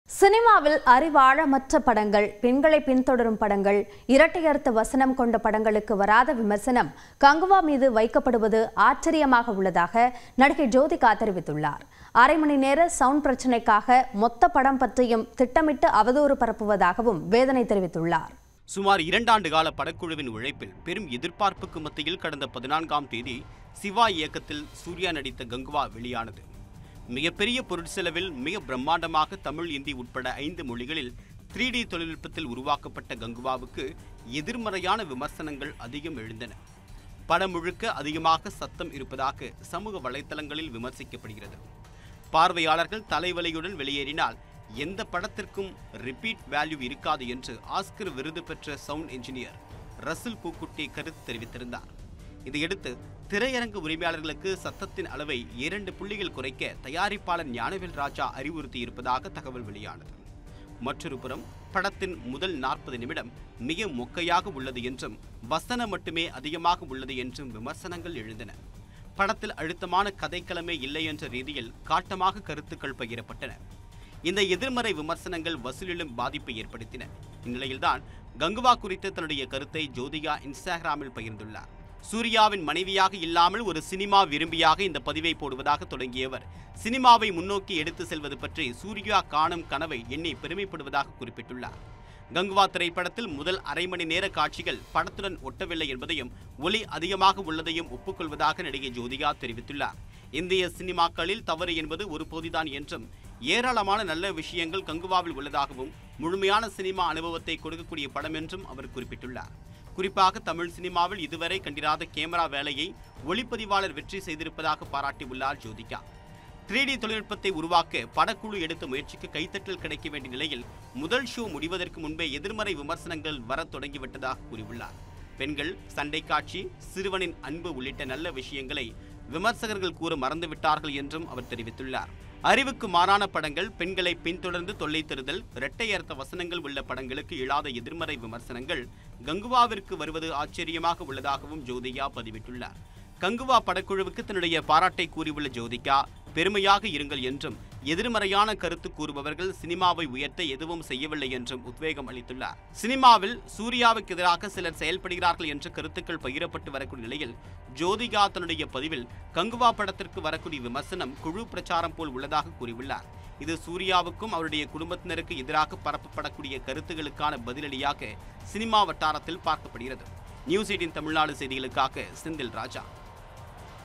Kanguva | அரை மணி நேரம் சவுண்ட் பிரச்னைக்காக முழு படத்தையும் விமர்சிப்பதா? - ஜோதிகா வேதனை
Kanguva | அரை மணி நேரம் சவுண்ட் பிரச்னைக்காக முழு படத்தையும் விமர்சிப்பதா? -  ஜோதிகா வேதனை | Kanguva Movie | Actor Suriya | Director Siva | News18 Tamil Nadu | Tamil News

18/11/2024 K